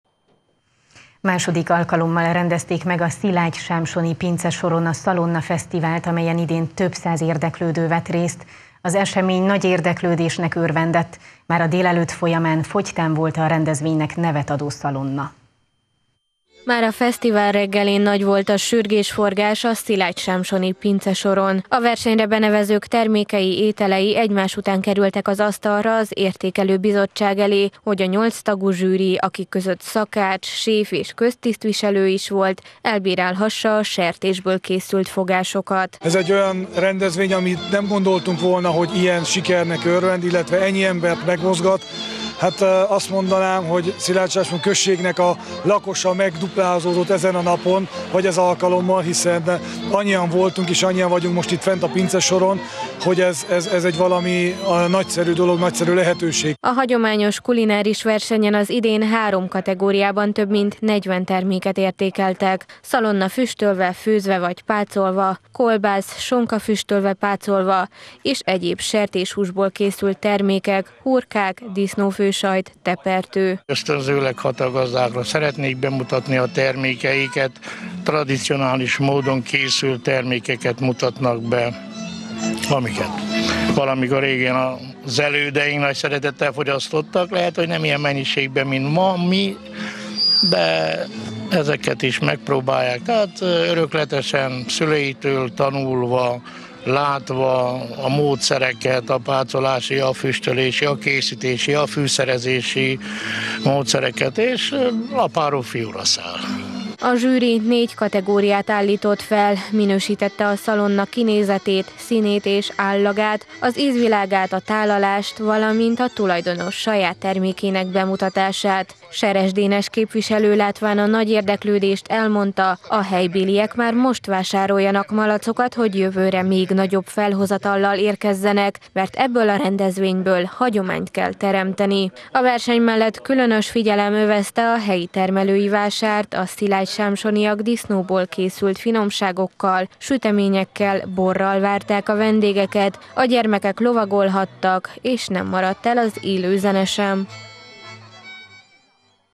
Szalonnafesztivál Szilágysámsonban – Erdélyi Magyar Televízió
Második alkalommal rendezték meg a Szilágysámsoni pincesoron a Szalonnafesztivált, amelyen idén több száz érdeklődő vett részt a megyéből és a szomszédos megyékből. Az esemény oly nagy érdeklődésnek örvendett, hogy már a délelőtt folyamán fogytán volt az eseménynek nevet adó szalonna.